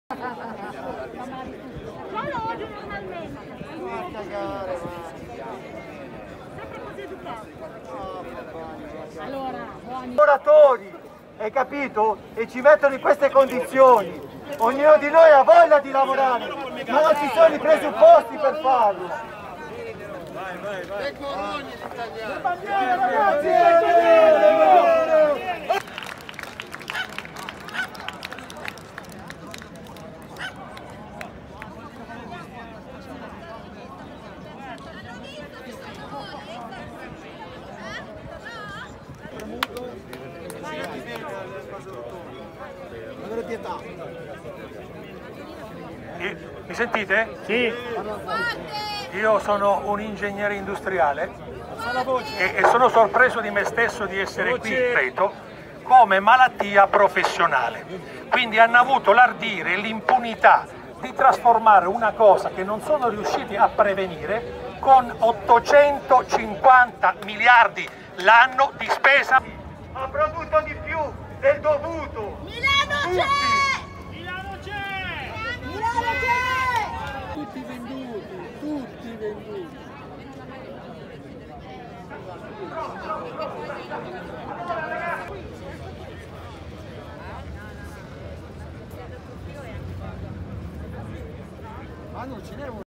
Taxi Drivers' Demonstration In Piazza Duomo In Milan
Taxi drivers' demonstration in Piazza Duomo in Milan to protest against the decrees of the Lombardy Region and the Italian Government, Milan May 16 2020. Italian taxi drivers are protesting about the economic measures that the Lombardy Region and the Italian Government have put in place for the restart after the coronavirus emergency. According to them, no economic aid has been provided in order to secure their activities, including the stop to taxes. (Footage by Mairo Cinquetti/NurPhoto)